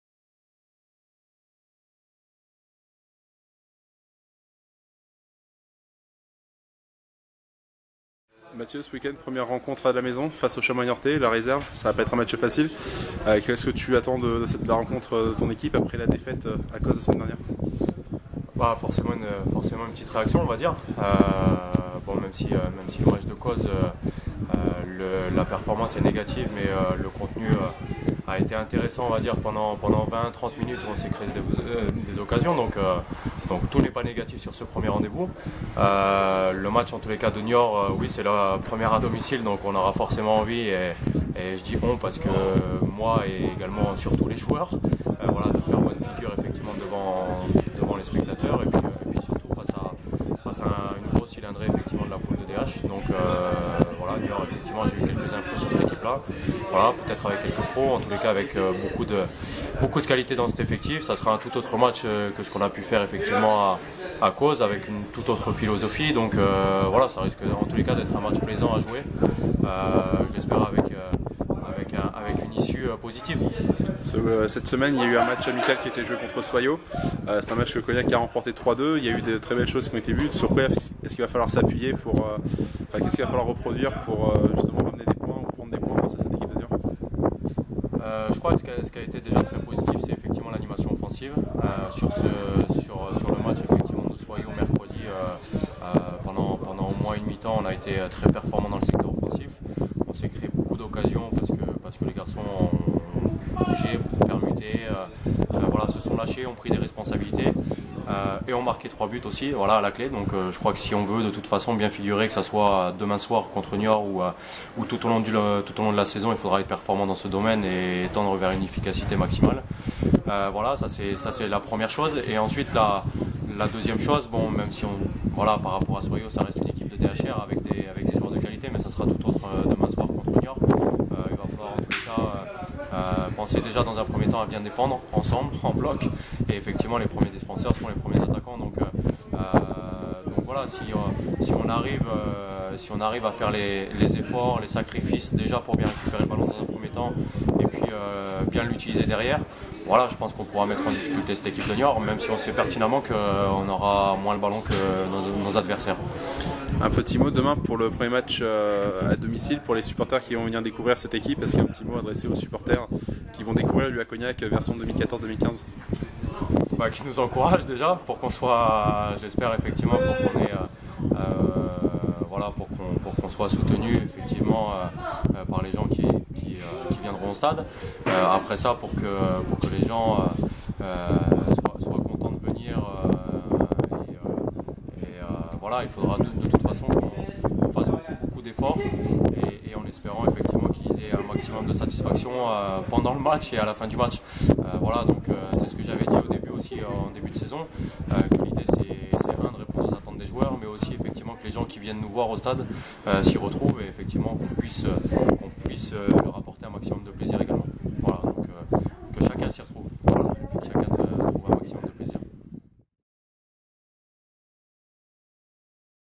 Présentation Cognac - Niort B
Mathieu Robin nous présente la rencontre UACF-Niort B comptant pour la deuxième journée du championnat DH.